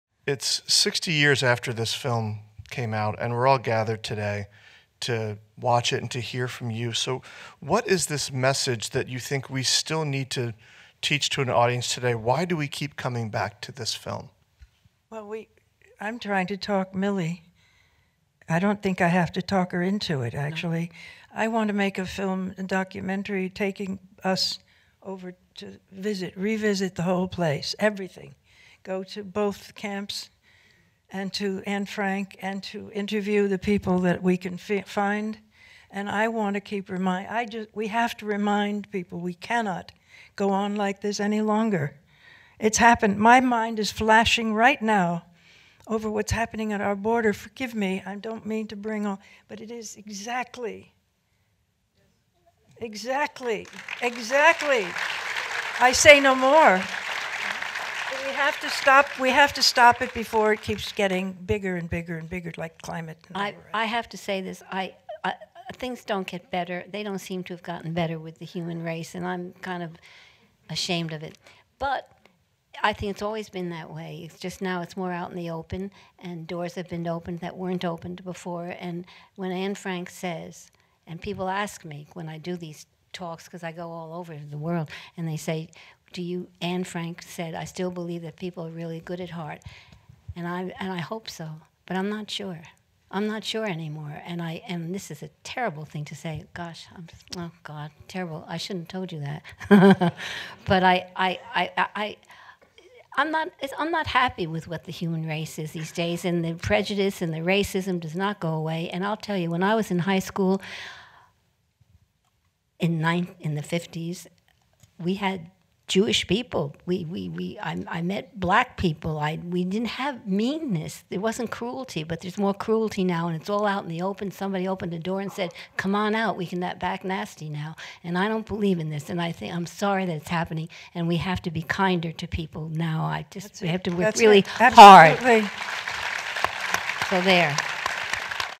"The Diary of Anne Frank": Sixty Years On
Actors Diane Baker and Millie Perkins explain the importance of "The Diary of Anne Frank" today at an Academy screening of 1959 film at the Samuel Goldwyn Theater in Beverly Hills.